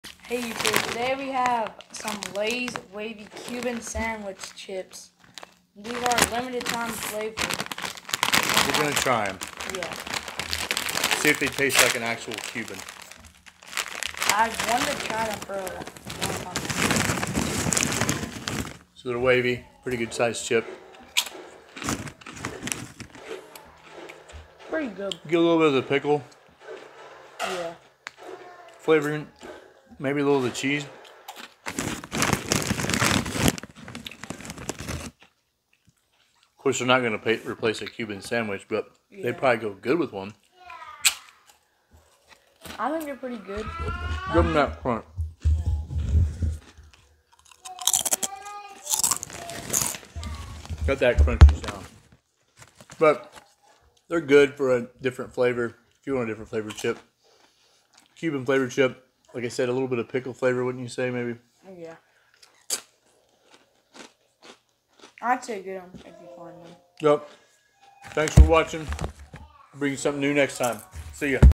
Lay's Cuban Flavored chips
Pretty good flavor. Obviously not just like a Cuban sandwich but definitely interesting.